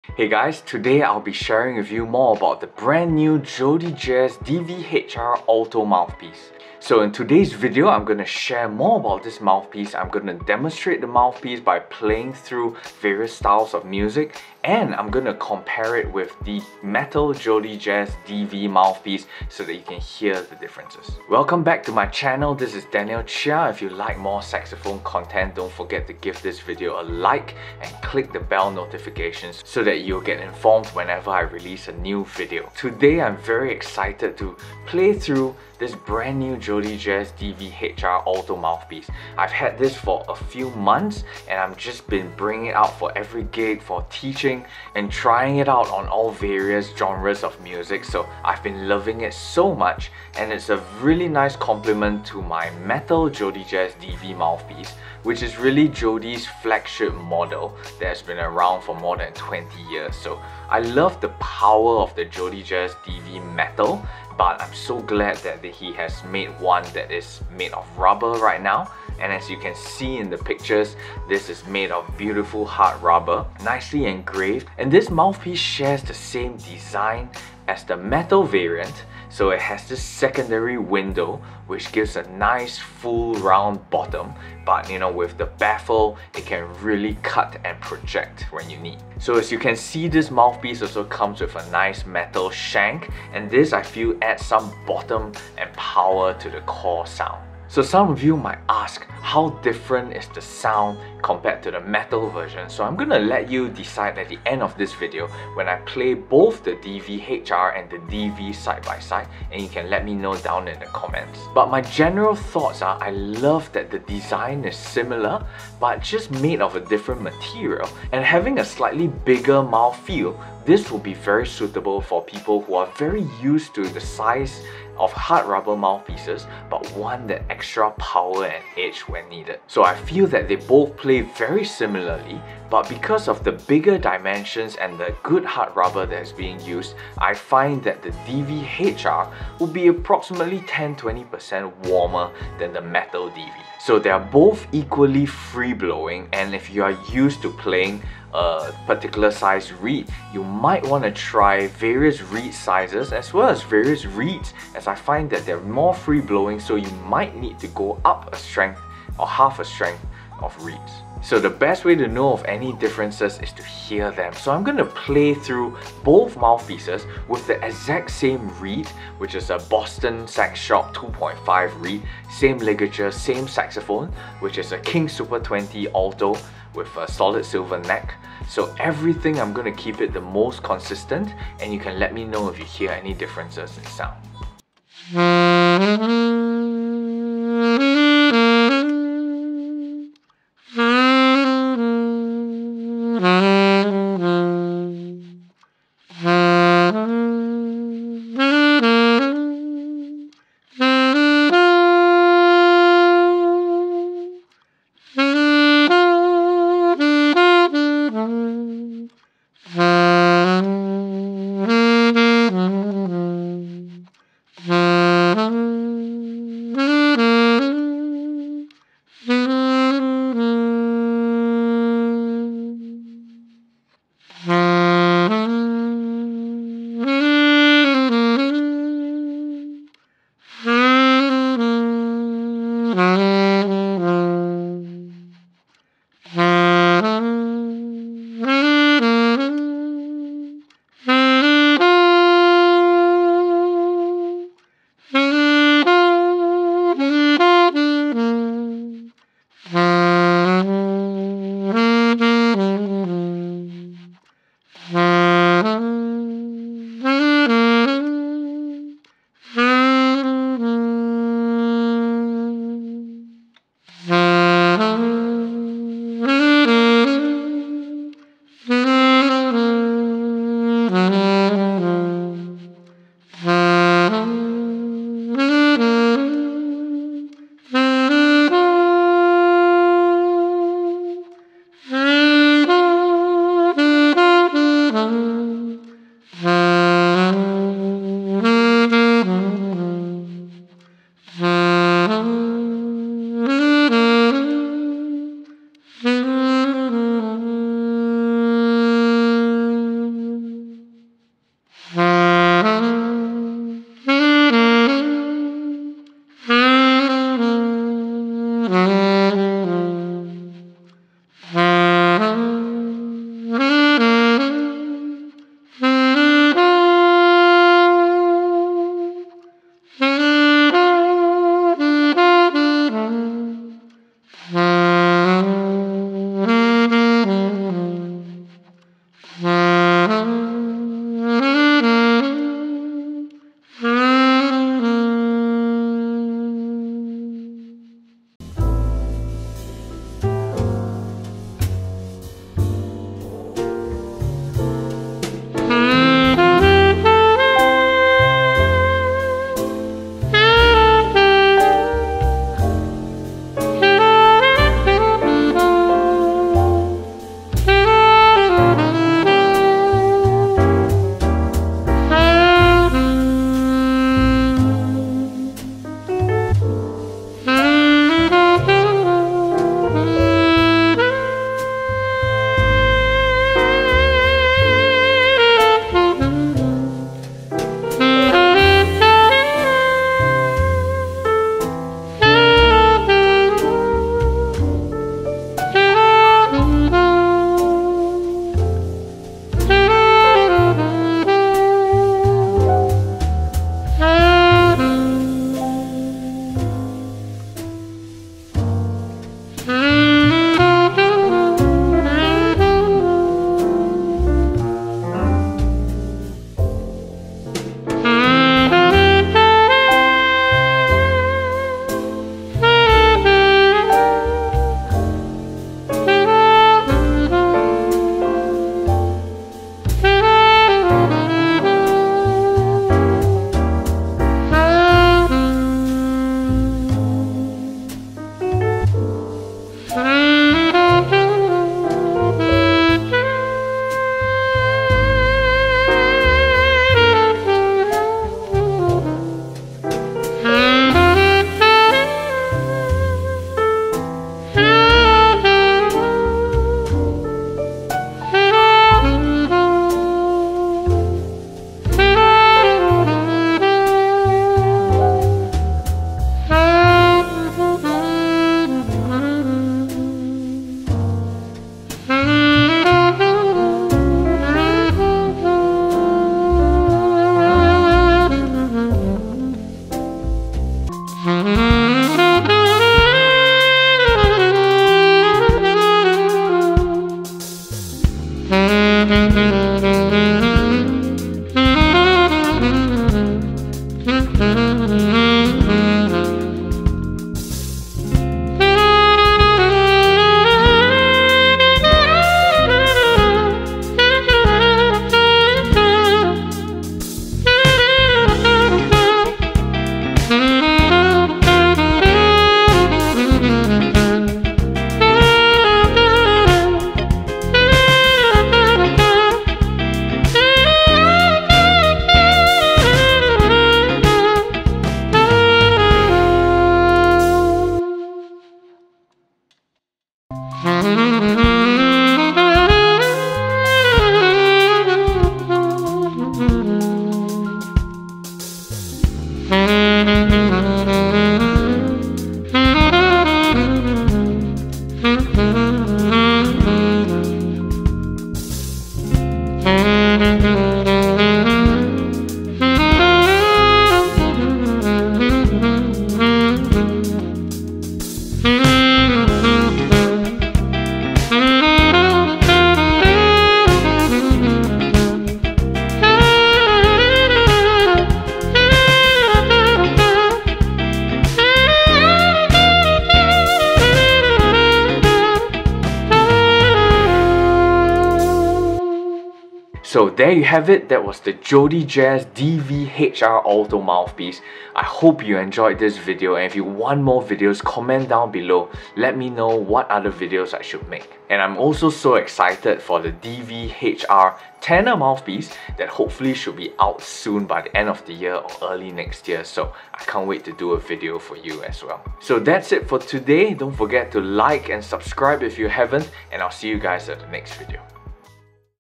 Is this WORTH the money? BRAND NEW JodyJazz DV HR Alto SAXOPHONE Mouthpiece
for all things SAX!

0:00 Introduction
0:33 About the DV HR
3:04 DV HR no reverb
3:44 DV no reverb
4:24 DV HR with reverb
5:02 DV with reverb
5:38 DV HR Jazz Ballad
6:47 DV Jazz Ballad
7:50 DV HR Funky
8:40 DV Funky
9:30 Final Thoughts

🎷All equipment here:
Saxophone: King Super 20
Mouthpiece: JodyJazz DV HR
Ligature: Ishimori Solid Silver
Reeds: Boston Sax Shop 2.5